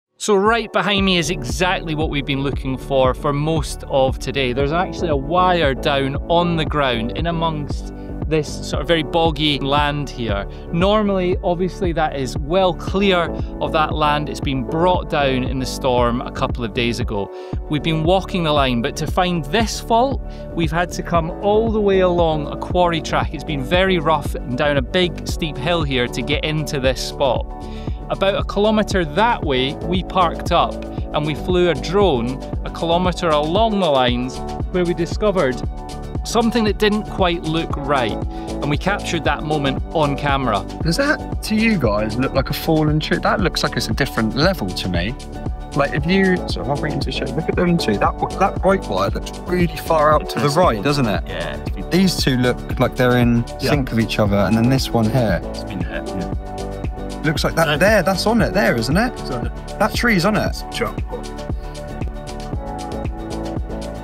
Drone spots hidden power line fault in storm!
After hours of searching on foot, we finally found the downed wire hidden deep in rough, boggy land. A quick drone flight along the line revealed what didn’t look right — saving us a tough trek.